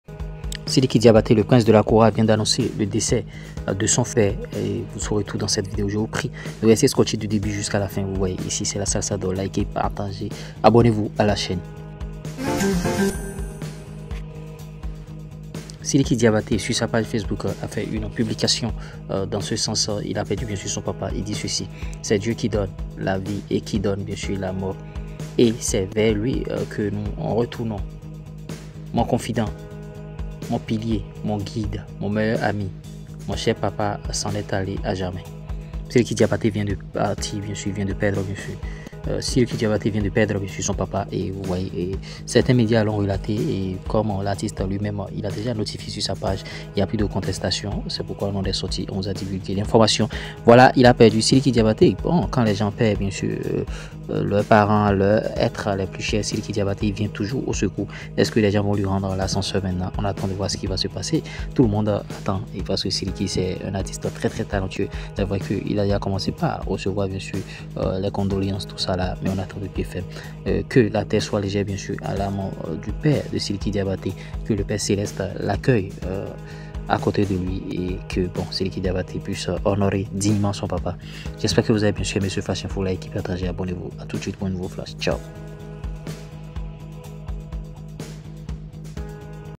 SIDIKI DIABATÉ PERD SON PAPA TOUMANI DIABATÉ
C'est avec une immense tristesse que nous avons appris le décès du maestro Toumani Diabate, père de notre cher Sidiki Diabaté. Toumani Diabaté, légende de la kora, a non seulement marqué la musique africaine de son talent inégalé .

✅ PAGES FACEBOOK : Lures Premier Officiel